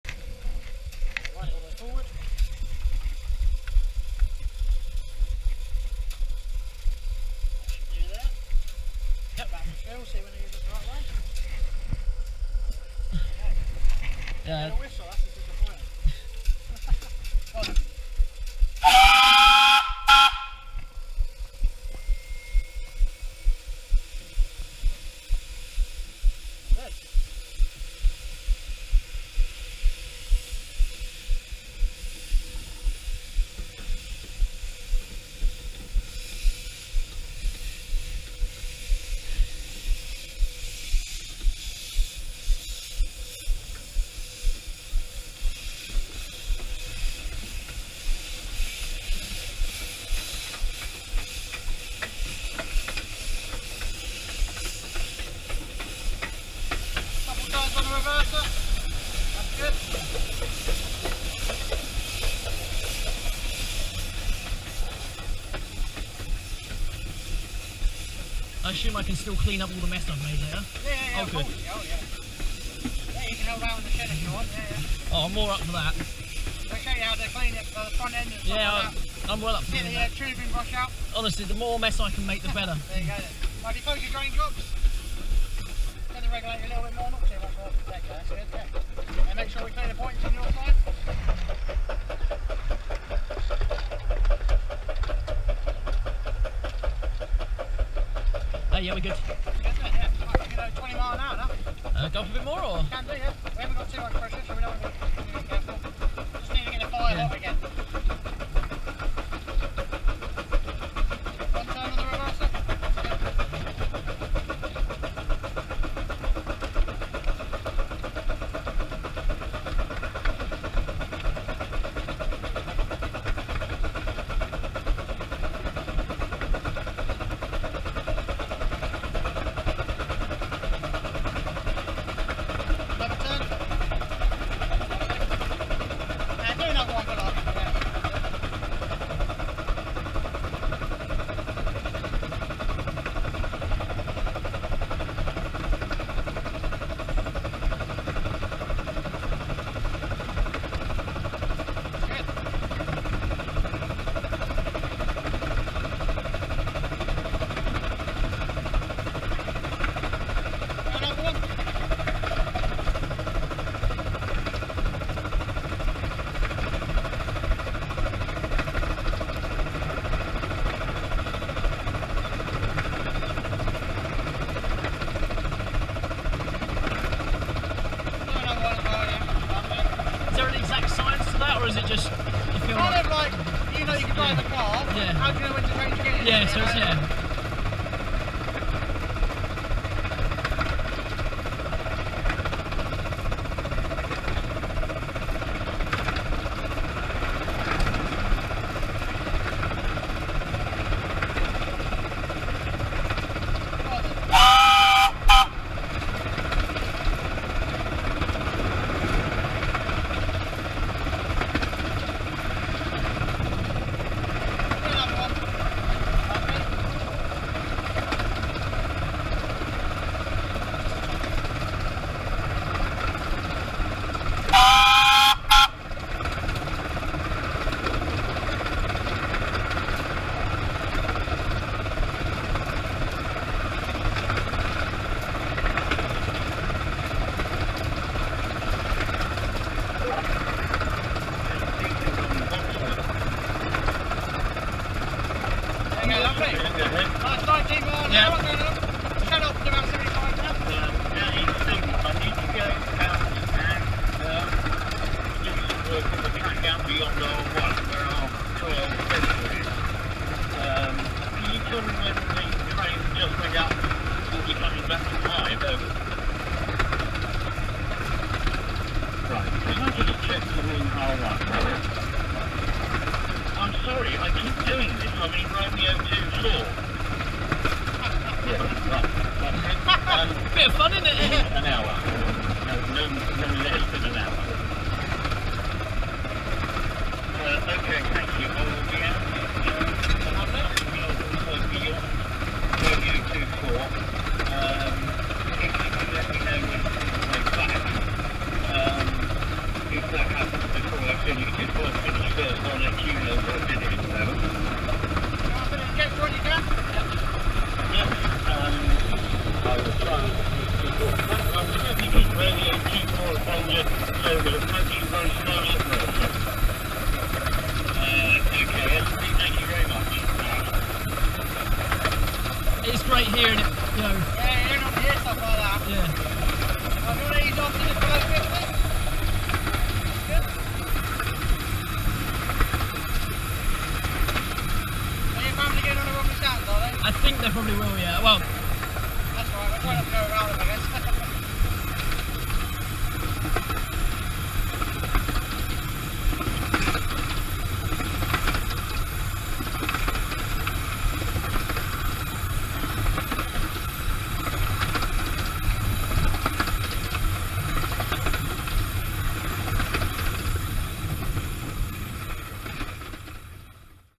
Romney, Hythe & Dymchurch Railway Driving Experience - Cab View (2022)
A view from the cab of Green Goddess driving from New Romney towards Dungeness.